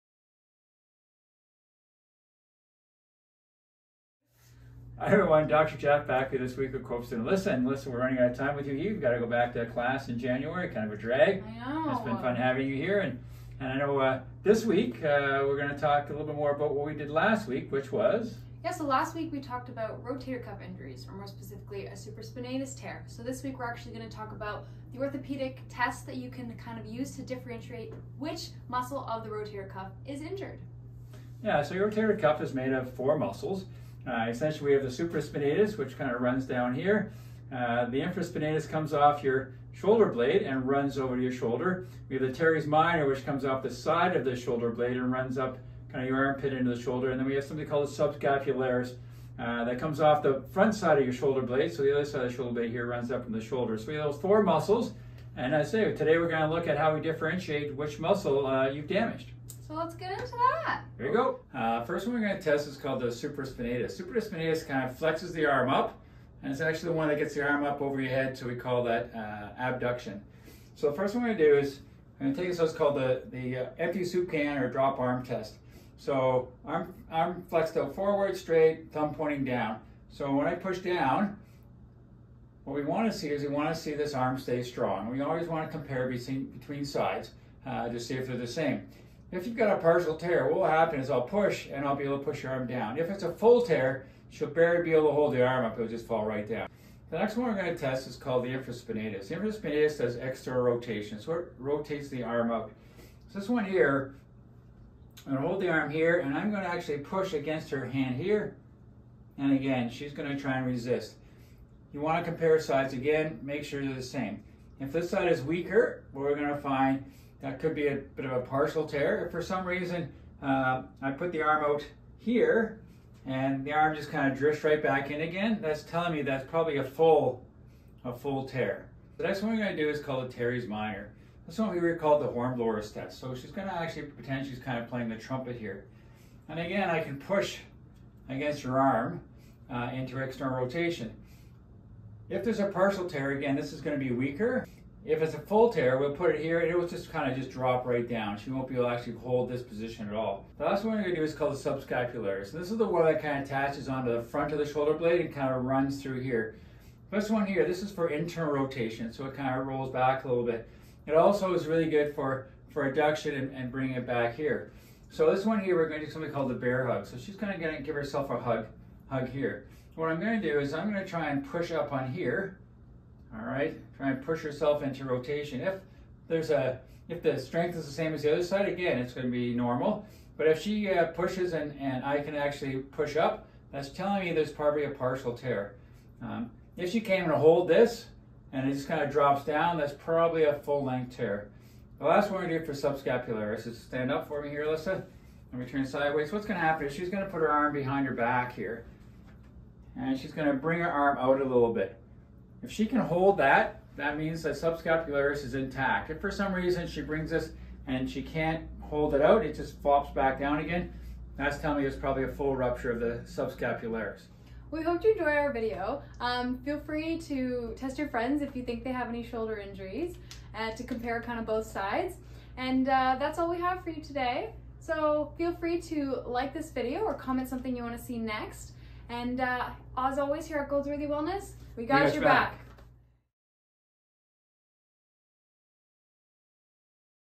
Orthopedic Tests for the Rotator Cuff
This video covers the orthopedic tests discussed in last weeks "Rotator Cuff Injuries" video, showing how to test each muscle to differentiate which one is torn.

During these tests, it's important to compare both sides in order to set a base line for the injured shoulder.
We hope you find these helpful and test your friends!